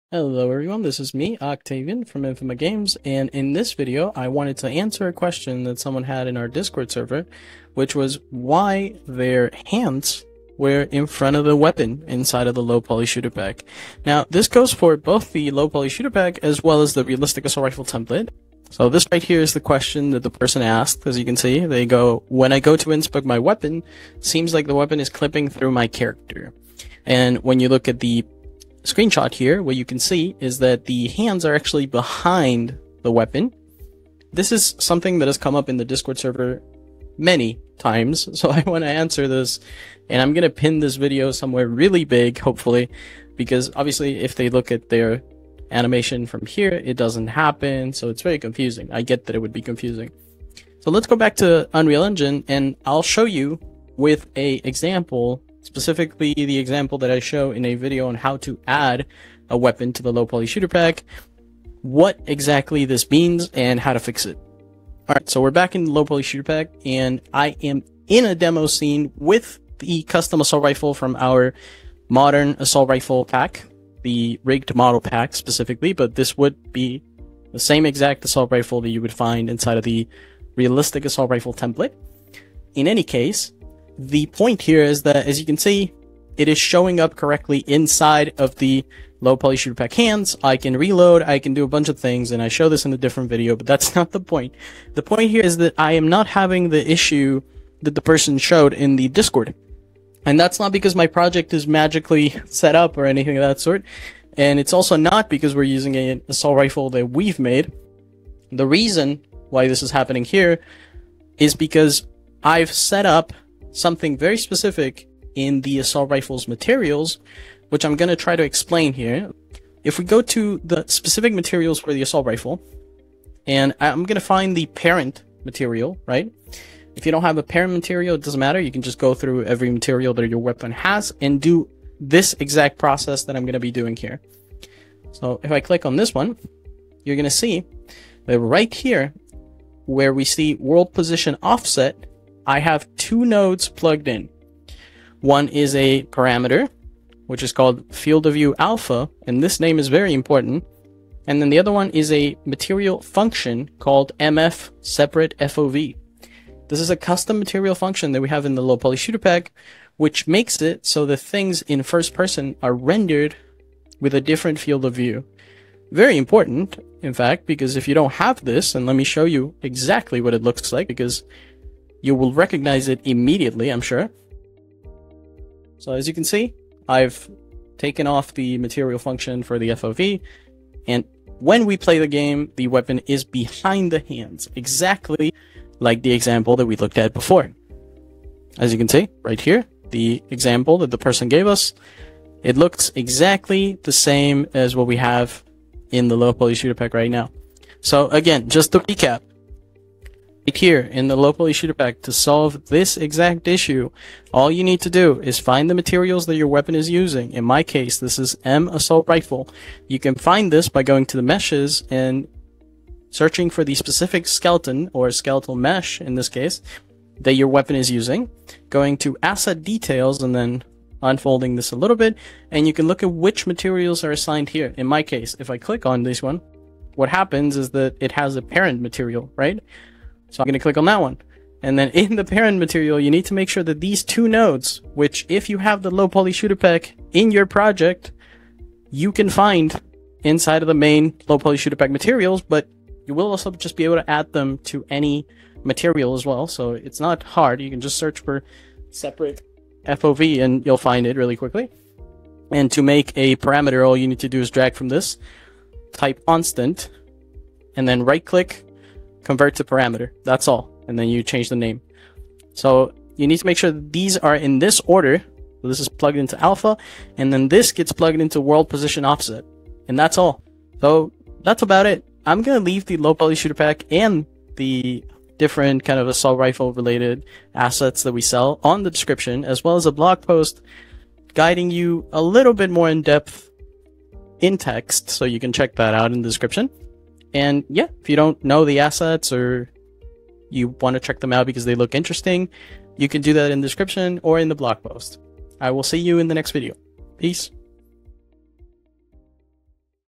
Fix Weapon Clipping in Unreal Engine | Low Poly Shooter Pack UE5
Hands appearing in front of your weapon when inspecting it in the Low Poly Shooter Pack? This is a common issue that happens when weapon materials aren't set up correctly for first-person rendering.

The weapon materials lack the MF_Sepparate_FOV material function, which renders first-person objects with a different field of view to prevent clipping.

1. How to identify the weapon clipping issue in first-person view
2. Where to find your weapon's materials and parent materials
3. How to add the FOV material function to world position offset
4. How to create and set up the FOV alpha parameter
5. Why this technique applies to any custom weapon in LPSP or RART

Created by Octavian Tocan and David Stenfors at Infima Games.

If you have questions or run into issues, drop them in the comments. I'll do my best to help.

*Chapters:*
00:00 Introduction - The Question
00:38 Showing the Problem
01:22 Setting Up Materials
02:06 The FOV Material Function
02:55 Before and After Comparison
03:35 Finding Your Weapon Materials
04:12 Step-by-Step Fix
04:55 Wrap Up